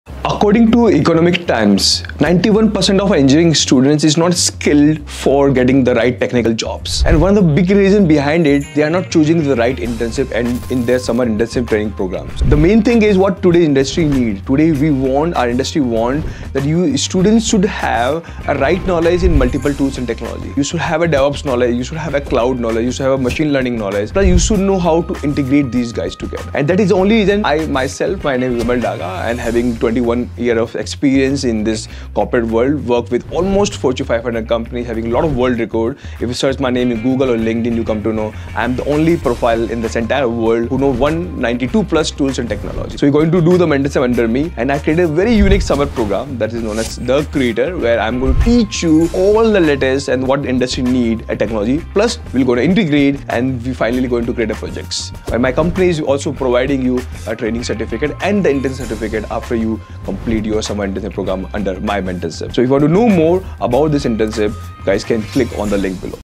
𝐅𝐨𝐫 𝐄𝐧𝐠𝐢𝐧𝐞𝐞𝐫𝐢𝐧𝐠 𝐒𝐭𝐮𝐝𝐞𝐧𝐭𝐬 - 𝐋𝐞𝐬𝐬 𝐂𝐆𝐏𝐀? 𝐇𝐚𝐯𝐢𝐧𝐠 𝐁𝐚𝐜𝐤𝐥𝐨𝐠𝐬? 😞 𝐒𝐭𝐢𝐥𝐥 𝐋𝐨𝐨𝐤𝐢𝐧𝐠 𝐟𝐨𝐫 𝐚 𝐉𝐨𝐛? 🧐
𝐋𝐞𝐬𝐬 𝐂𝐆𝐏𝐀?  𝐇𝐚𝐯𝐢𝐧𝐠 𝐁𝐚𝐜𝐤𝐥𝐨𝐠𝐬? 😞
𝐒𝐭𝐢𝐥𝐥 𝐋𝐨𝐨𝐤𝐢𝐧𝐠 𝐟𝐨𝐫 𝐚 𝐉𝐨𝐛? 🧐

𝐓𝐡𝐞𝐧 𝐉𝐨𝐢𝐧 🤩 𝐋𝐖'𝐬 𝐒𝐮𝐦𝐦𝐞𝐫 𝐈𝐧𝐭𝐞𝐫𝐧𝐬𝐡𝐢𝐩 𝐈𝐧𝐝𝐮𝐬𝐭𝐫𝐢𝐚𝐥 𝐓𝐫𝐚𝐢𝐧𝐢𝐧𝐠 𝐏𝐫𝐨𝐠𝐫𝐚𝐦 - 𝐓𝐡𝐞 𝐂𝐫𝐞𝐚𝐭𝐨𝐫 𝟐𝟎𝟐𝟒

Summer Internship Program under 𝐖𝐨𝐫𝐥𝐝 𝐑𝐞𝐜𝐨𝐫𝐝 𝐇𝐨𝐥𝐝𝐞𝐫 𝐌𝐫. 𝐕𝐢𝐦𝐚𝐥 𝐃𝐚𝐠𝐚 who Empowers 1000+ Engineering Students to Innovate and Collaborate

This internship is more than just a learning experience; 𝒊𝒕'𝒔 𝒂𝒏 𝒊𝒏𝒗𝒊𝒕𝒂𝒕𝒊𝒐𝒏 𝒕𝒐 𝒋𝒐𝒊𝒏 𝒕𝒉𝒆 𝑳𝒆𝒂𝒈𝒖𝒆 𝒐𝒇 𝑪𝒓𝒆𝒂𝒕𝒐𝒓𝒔.

So, if you're ready to take the leap and become a part of the League of Creators, seize this opportunity with both hands.

𝐔𝐧𝐢𝐪𝐮𝐞 𝟕 𝐒𝐮𝐦𝐦𝐞𝐫 𝐏𝐫𝐨𝐣𝐞𝐜𝐭𝐬 𝐨𝐧:
🎯 Generative AIOps
🎯 Artificial Intelligence with ML / DL
⁠🎯 Cloud Computing
🎯 MLOps & DevOps
🎯 Full Stack Development
🎯 Linux & Python Programming
🎯 Metaverse & Industry 4.0

Starting from : 10th May / 5th June/ 15th July, 2024
Duration : 6 weeks / 8 weeks

For any query, get in touch with our Summer Team on 📞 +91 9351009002 | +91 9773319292